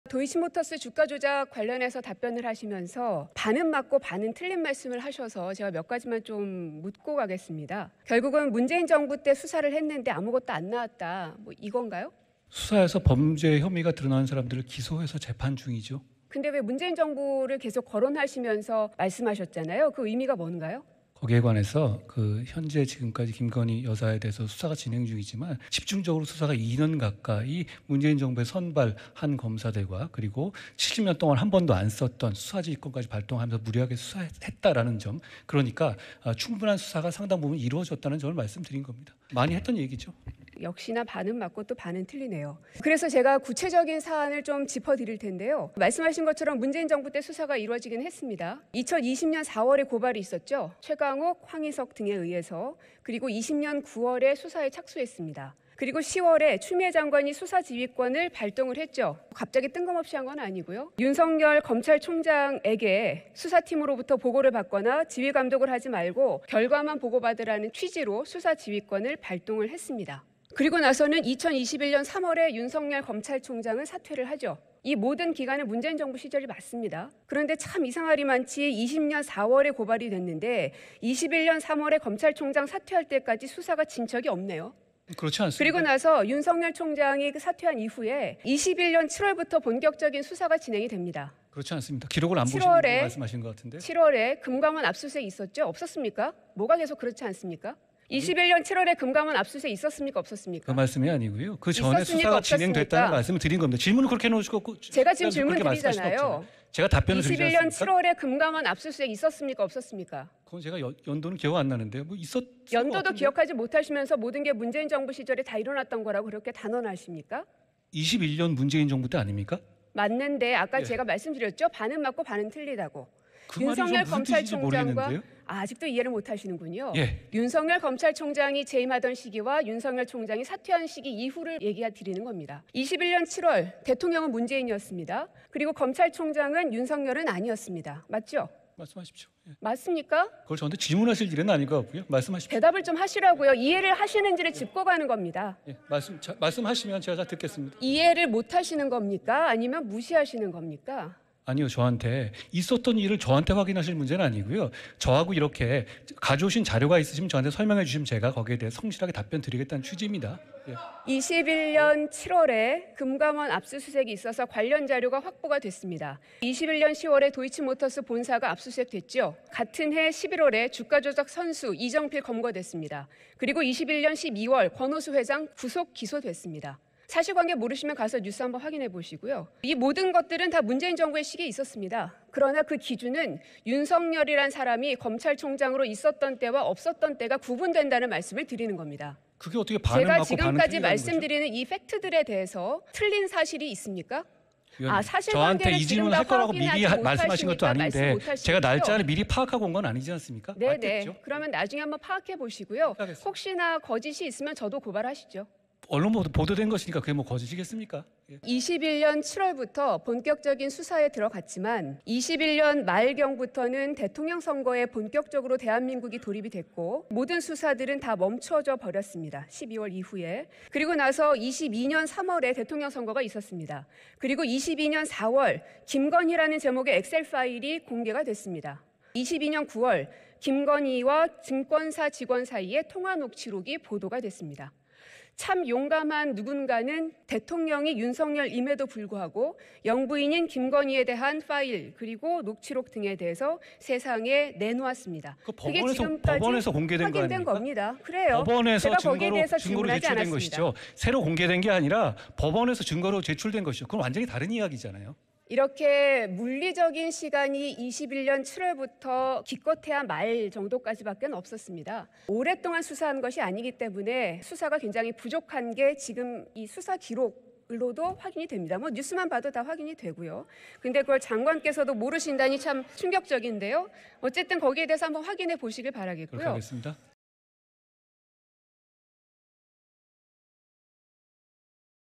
'도이치모터스 주가조작' 시간순으로 정리한 고민정 "틀린 거 있으면 나도 고발하던지ㅋ"
230206 국회 대정부질문